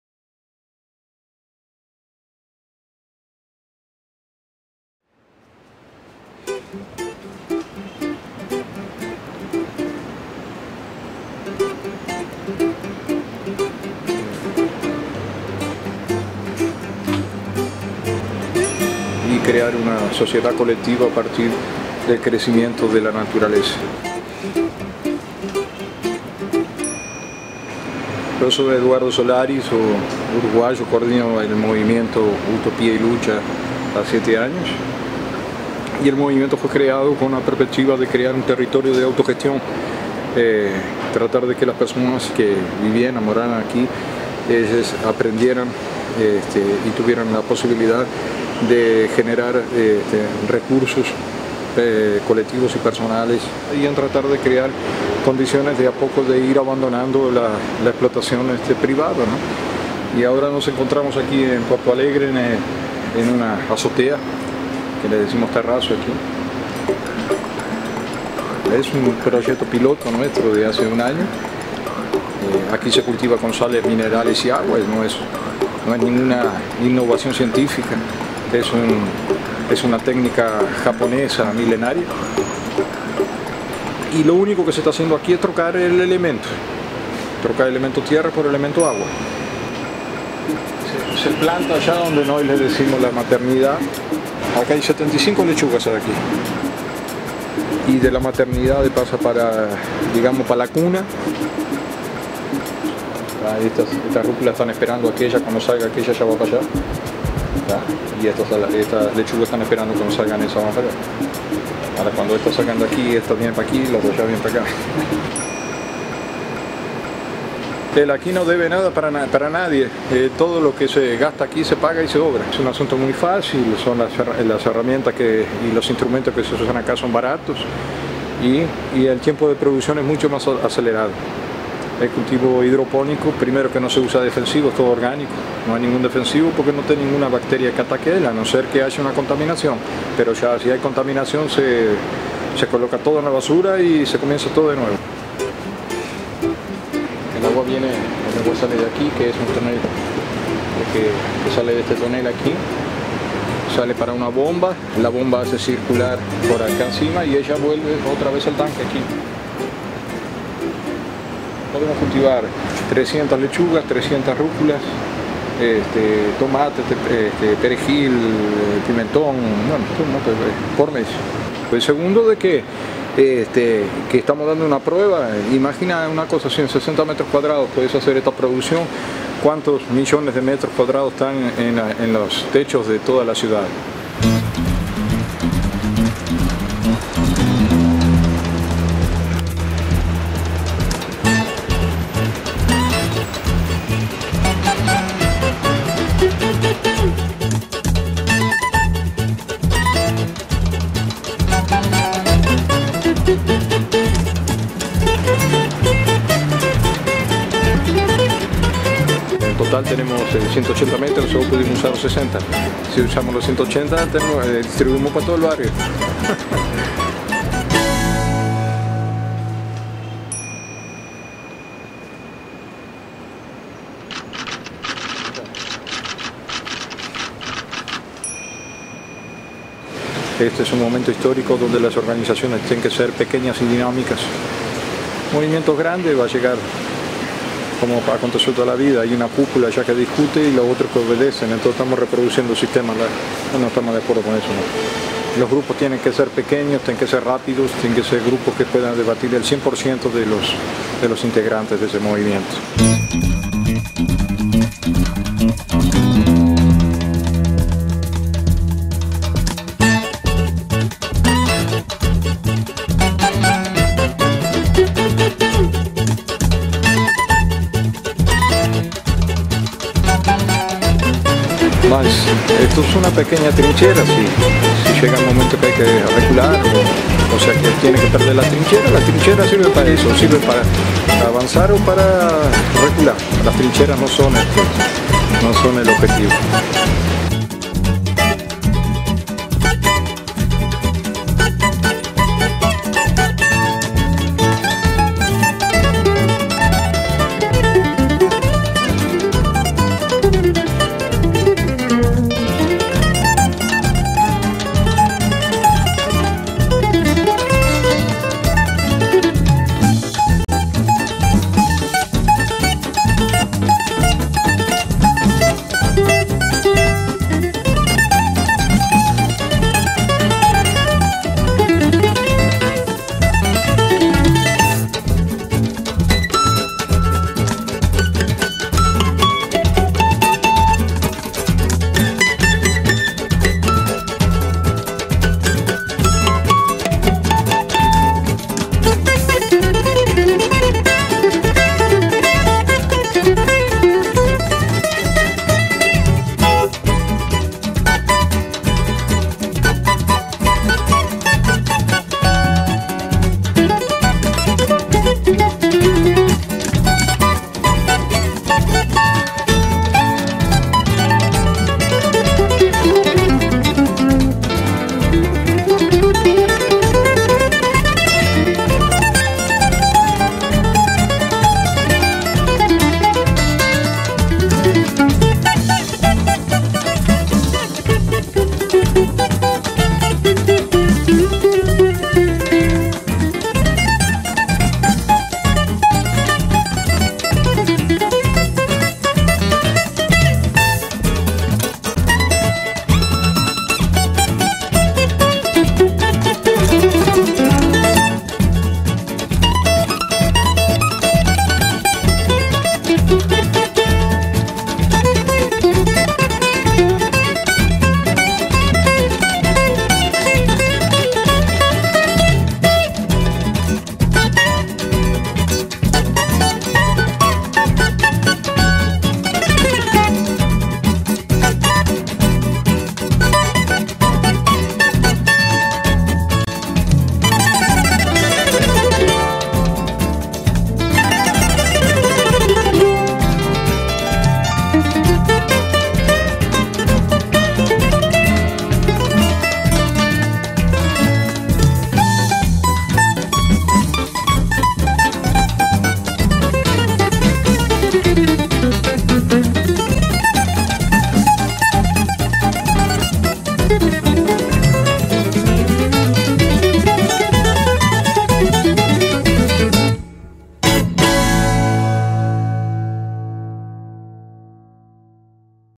Plante y Luche - Como realizar cultivos hidropónicos- Movimiento Utopia e Luta
Hidroponia - Agricultura Urbana - (Duracion: 4.30 min)
Utopia Verde, en la ciudad de las furias,. Un edificio, un proyecto, un sueño.
Comunidad  Autonoma Utopia e Luta
Compartimos con Eduardo, una tarde en la terraza de un edificio tomado en el centro del Puerto Caos Alegre, en Brasil. Gracias por el trato, la compañia y todo el saber, a difundir ...